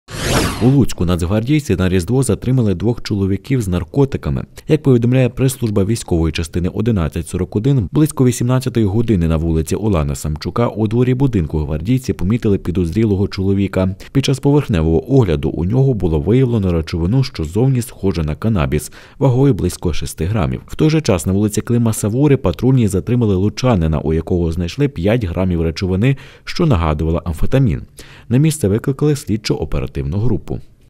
У Луцьку нацгвардійці під час патрулювання затримали двох чоловіків з наркотиками
У Луцьку нацгвардійці на Різдво затримали двох чоловіків з наркотиками. Близько 18 години на вулиці Улана Самчука, у дворі будинку гвардійці помітили підозрілого чоловіка.